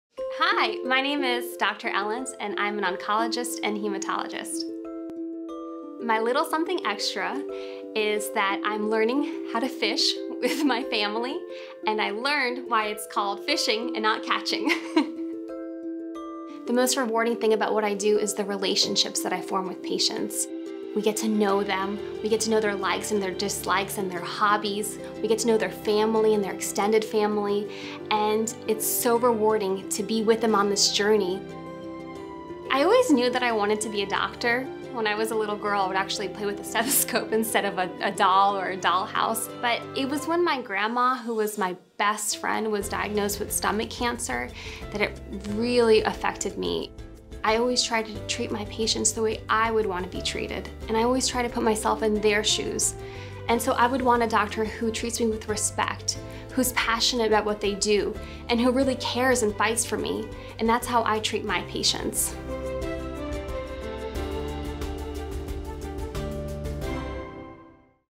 Meet Dr. Ellent! Cancer Care at West Jefferson Medical Center
Get to know Dr. Elizabeth Ellent, an Oncologist with West Jefferson Medical Center.